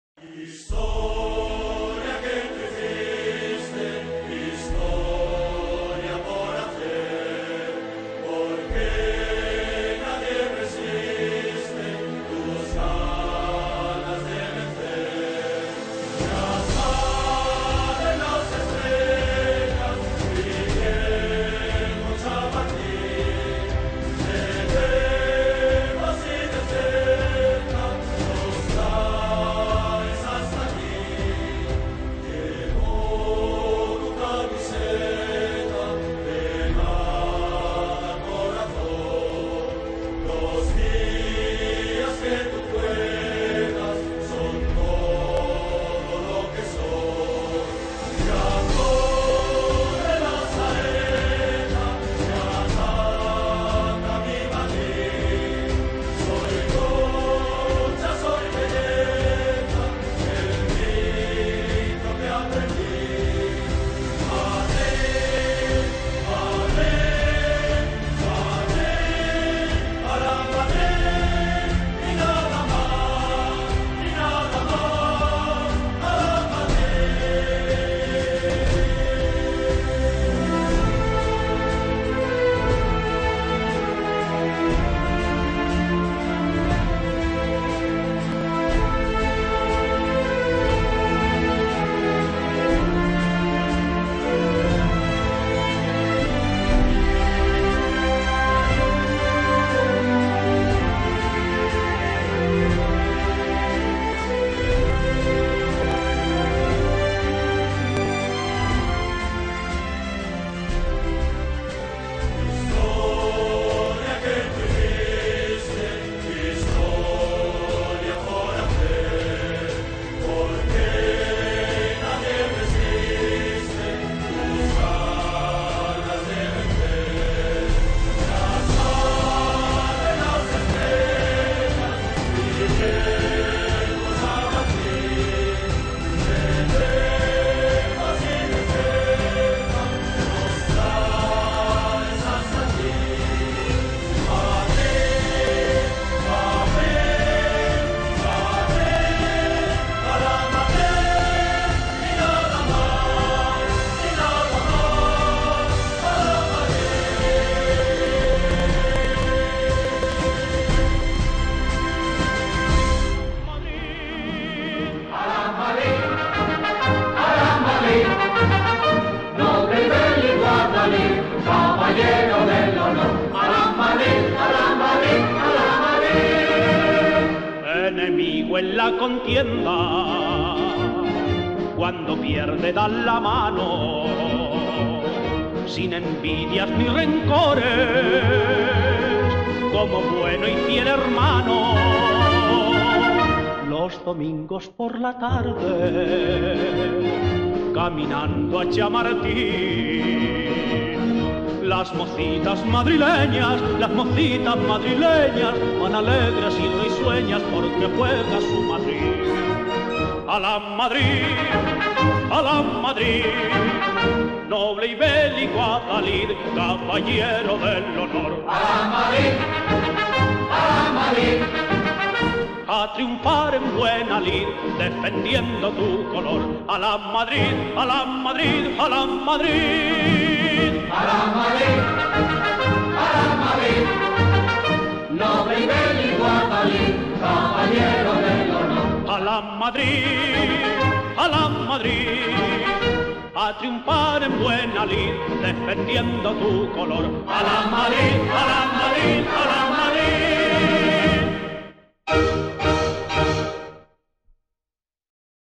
PM Miguelturra Fotografías 25 aniversario
Fotografías 25 aniversario PM Miguelturra con la asistencia de los ex jugadores Francisco Bonet y Juan José Jiménez en representación del Club.